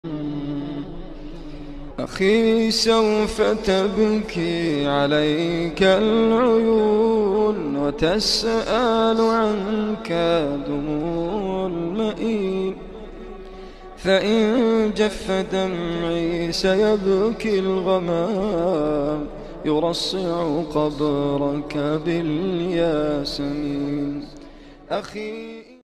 After ten years, the body of Afghan Taliban commander Obaidullah alias Zarqawi was exhumed
When the commander of the Afghan Taliban, Obaidullah alias Zarqawi, was moved due to the flood, he was taken out of the grave after 10 years as if he had been buried today.  In reality, the promise of my Allah is true.